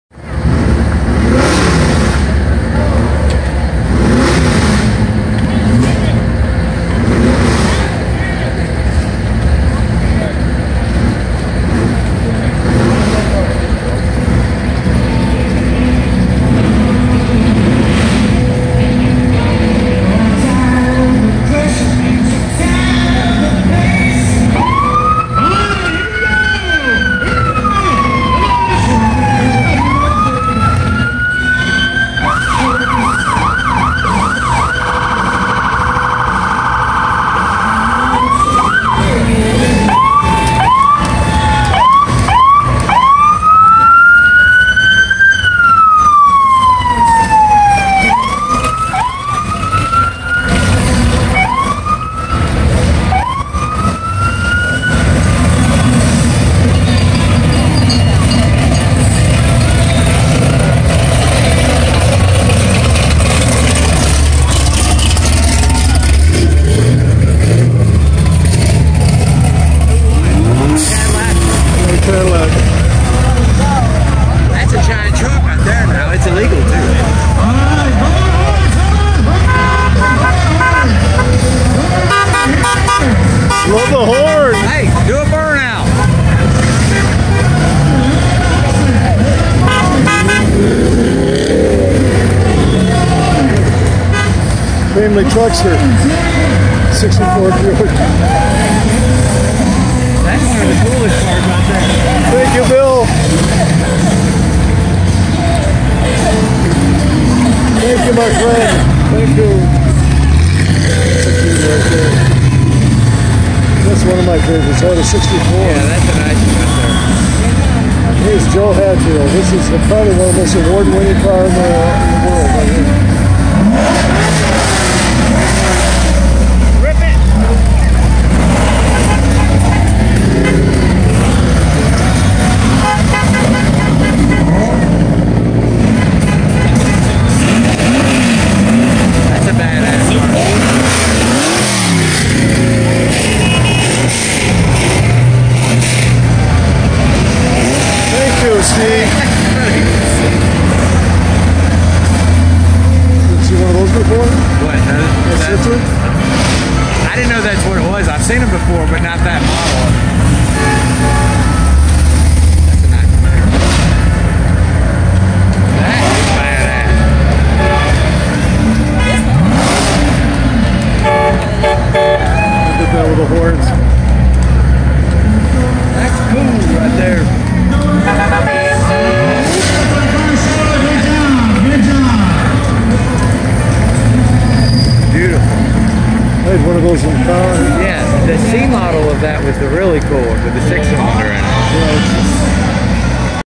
2017 Peachtree Corners Festival South East Wheels Events Car & Bike Show Sunday "Drive-Out"
This video is the Sunday June 11, 2017 Drive-Out! The Peachtree Corners Festival was again a great success. The South East Wheels Events Car and Bike Show and the parade like "Drive-Out" each day has become a big part of the Festival tradtitions.
For more great events like this visit your regional calendar of events, and UKWheelsEvents.com